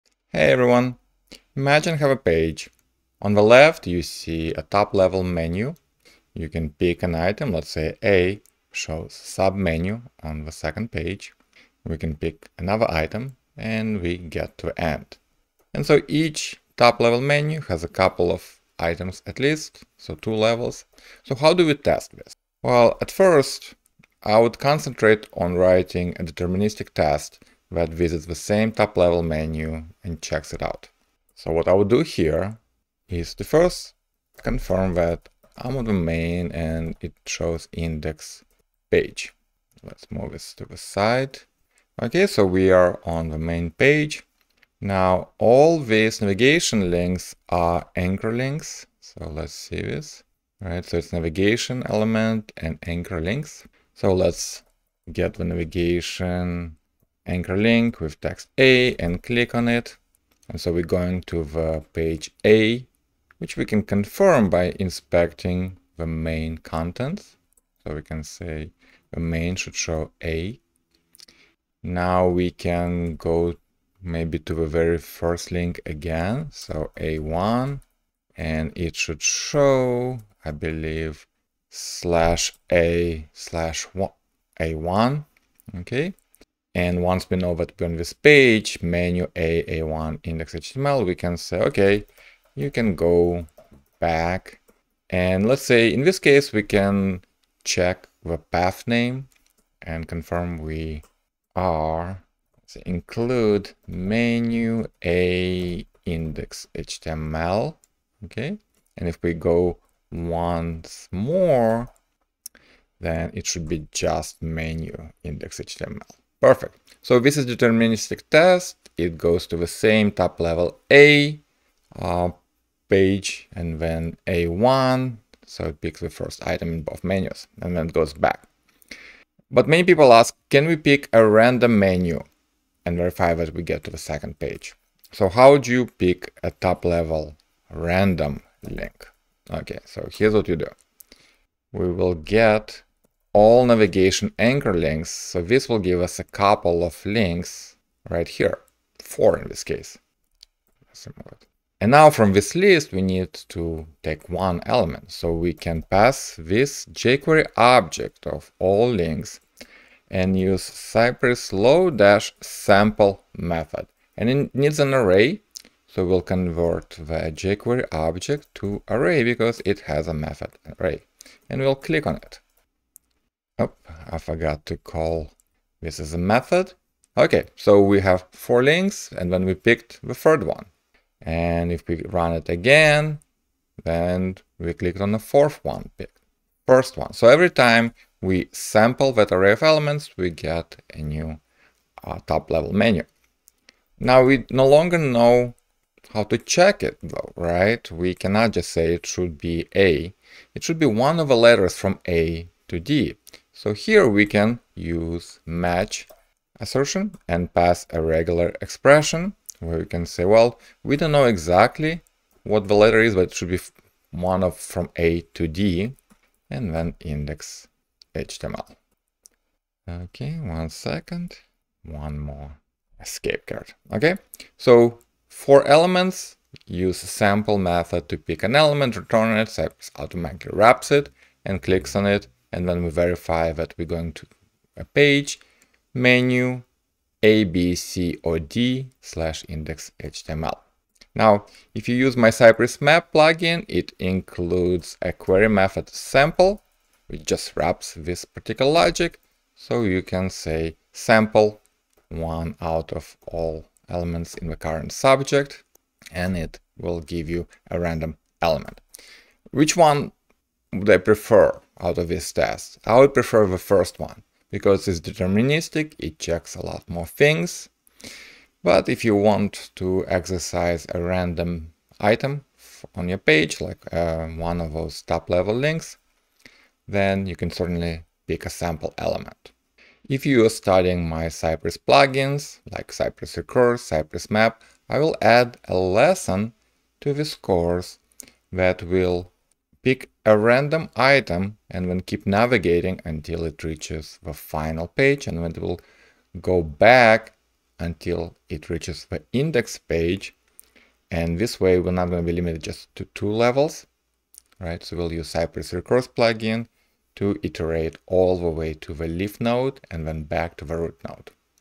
Pick A Random Menu Link
This video shows how to pick a random menu navigation link and click on it and check if we are navigating to the page.
cy.contains('main', 'Index page')
// cy.sample comes from cypress-map plugin
cy.get('nav a').sample().click()
cy.location('pathname').should(
  'match',
  /\/menu\/[a-d]\/index\.html/,
If you cannot use cypress-map plugin, you can still pick a random element using the following code:
cy.get('nav a')
  .then(function ($a) {
      return Cypress._.sample($a.toArray())
  .click()
repo. Find more exercises in my Cypress Plugins course (see the lesson "Lesson i8: Randomly pick a menu")